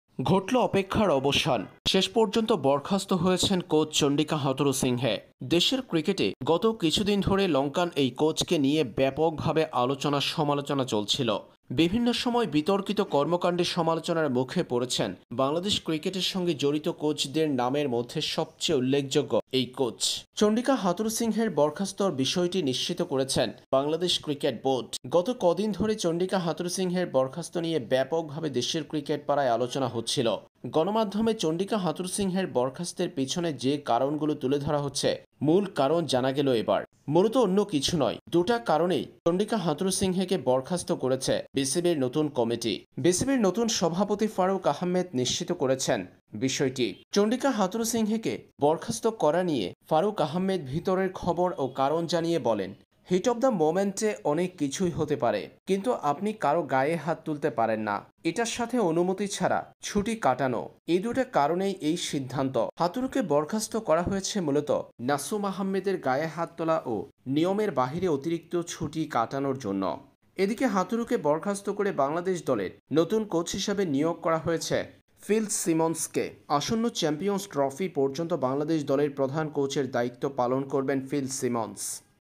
মিডিয়ায় যা শুনছেন তা নয়, মূলত যেকারণে বর*খাস্ত হলো হাথুরুসিংহে? বেরিয়ে এলো ভয়*ঙ্কর তথ্য? জানুন | bcb
Bangladesh National Cricket Team Head Coach Chandika Hathurusingha has been suspended. Bangladesh cricket team new coach is phil simmons.  Bangladesh cricket board BCB Announced new head coach name, He is phil simmons.